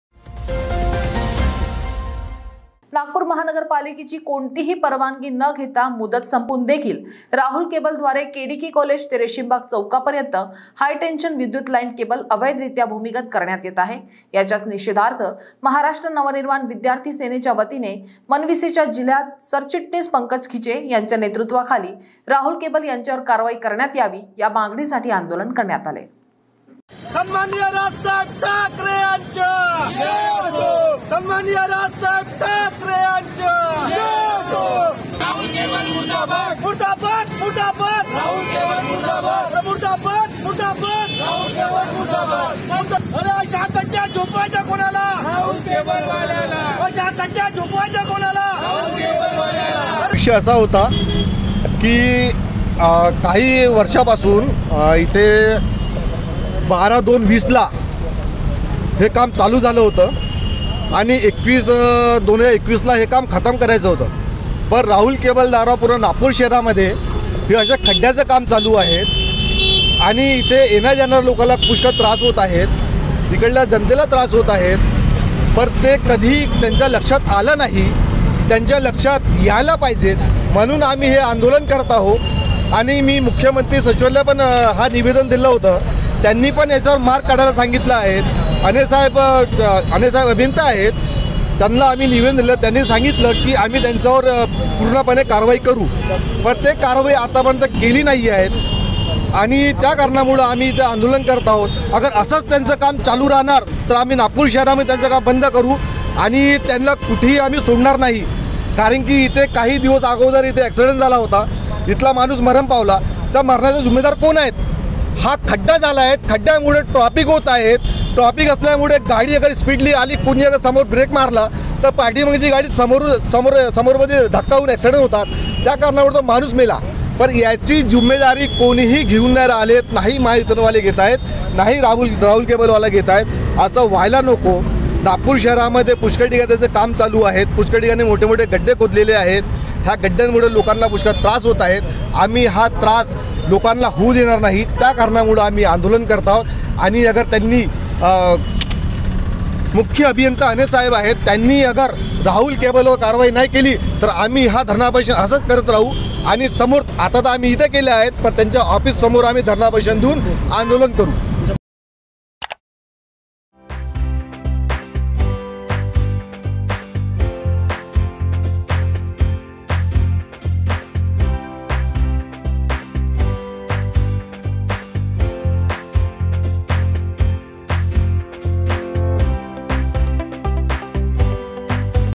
मनविसे च्या वतीने राहुल केबल विरोधात आंदोलन
नागपूर महानगरपालिकेची कोणतीही परवानगी न घेता मुदत संपून देखील राहुल केबल द्वारे KDK कॉलेज ते रेशीमबाग चौकापर्यंत हायटेंशन विद्युत लाईन केबल अवैधरित्या भूमिगत करण्यात येत आहे. याच्याच निषेदार्थ महाराष्ट्र नवनिर्माण विद्यार्थी सेनेच्या वतीने मनविसे च्या जिल्हा सरचिटणीस पंकज खिचे यांच्या नेतृत्वाखाली राहुल केबल यांच्यावर कारवाई करण्यात यावी या मागणीसाठी आंदोलन करण्यात आले.